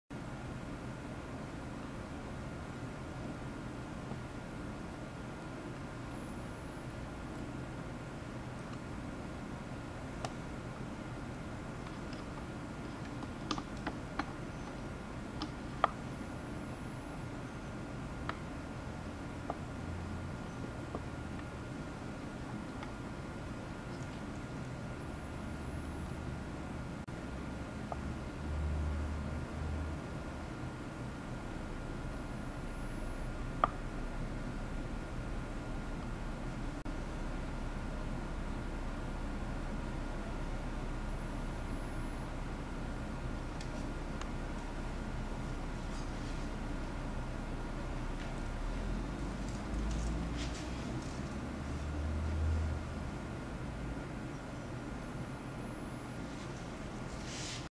Anole on the make
Last year in August we noticed an Anole visited us and got a few pictures.   Today 9/1/2014, an another anole visited.  Possibly the same one as last year.   I believe the motion he does on the tree is a type of mating call. - location Juno Beach, FL.